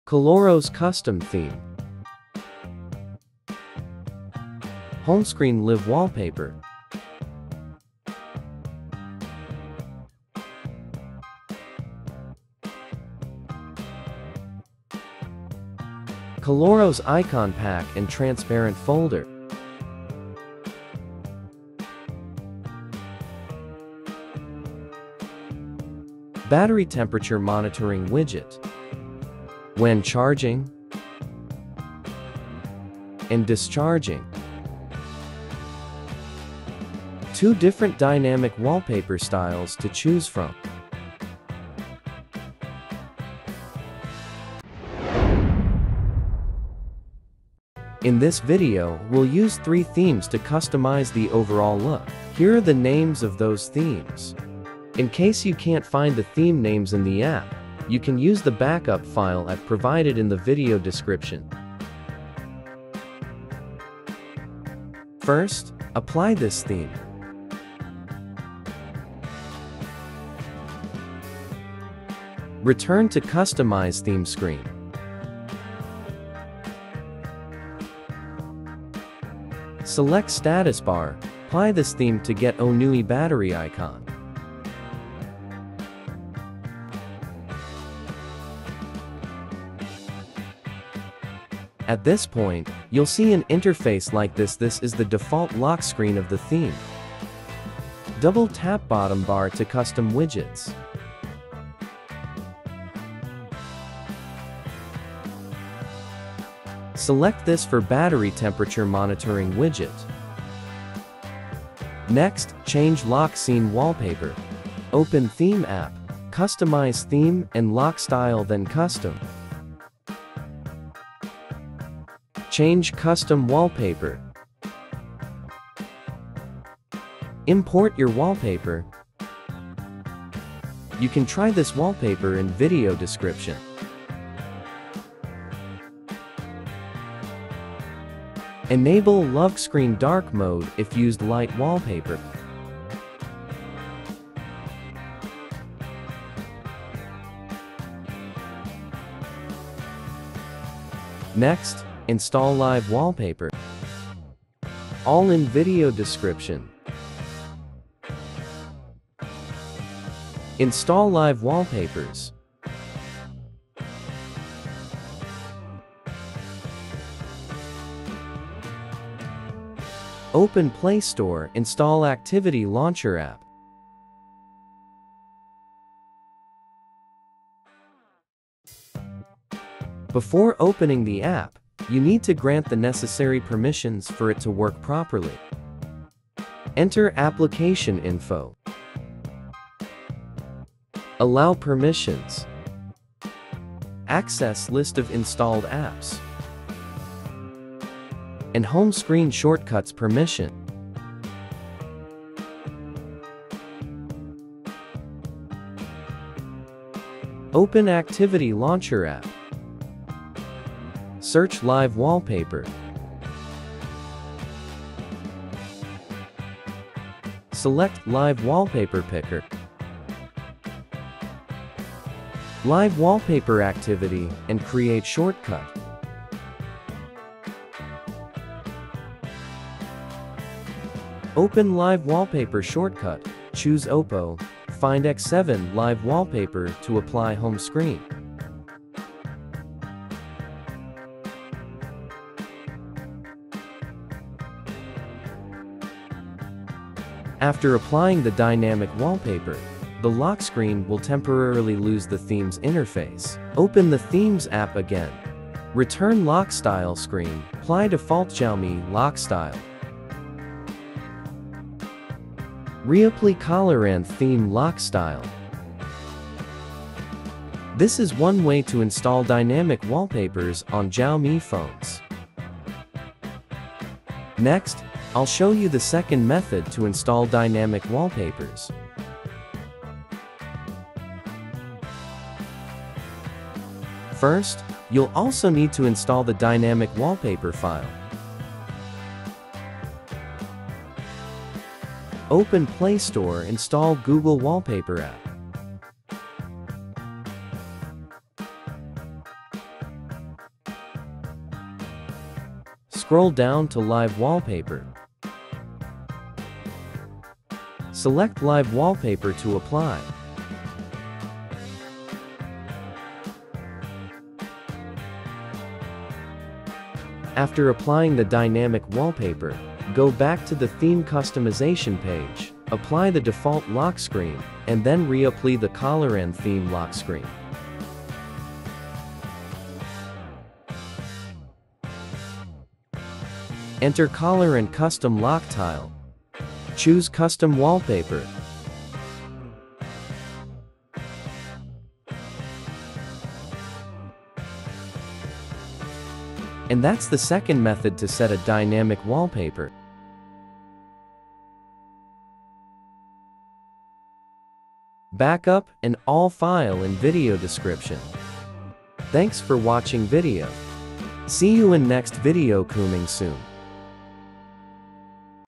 ColorOS Custom Theme With Dynamic Wallpaper For Xiaomi HyperOS
Bring the ColorOS-inspired custom theme to your Xiaomi HyperOS device with a dynamic wallpaper that reacts beautifully to your home screen setup. Smooth animations, refined icons, and full system compatibility make your Xiaomi look premium and unique.
Backup:
How To Install Chinese Themes On Xiaomi Global From Backup File - HyperOS & MIUI